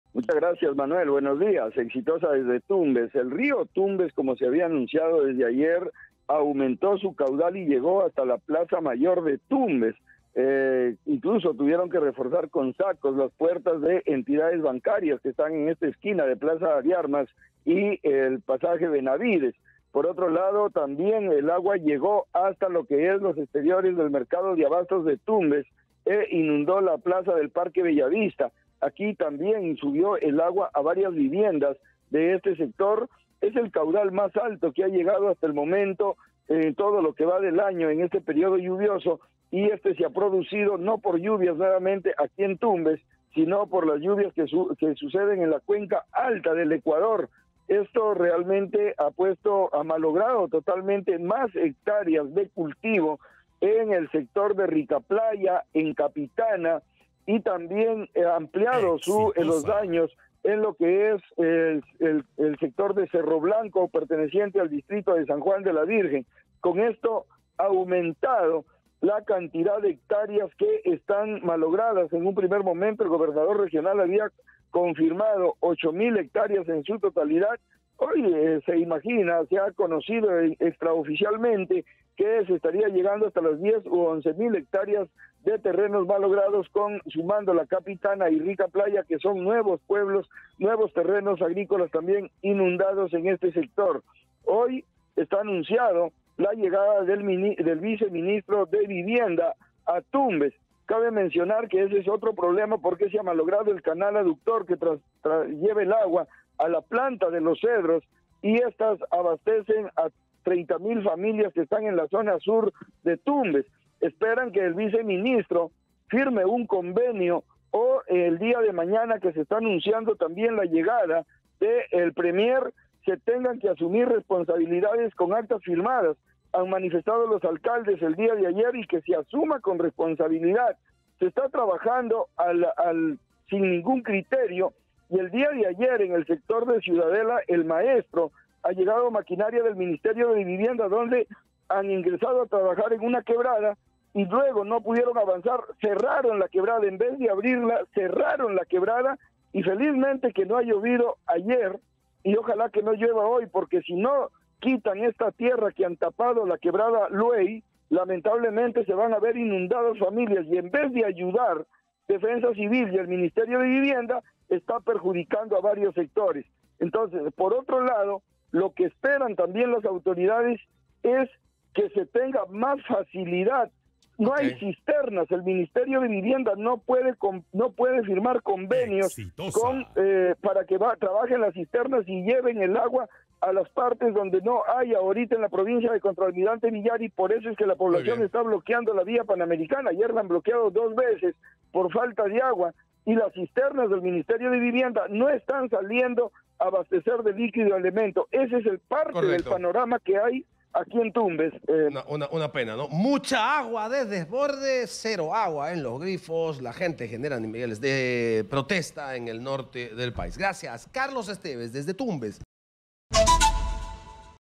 🔴🔵Reportan aumento de caudal del río Tumbes
Equipo de Exitosa reporta el aumento de caudal del río Tumbes.

Exitosa Perú
"Exitosa: La voz que integra al Perú"

Corporación Universal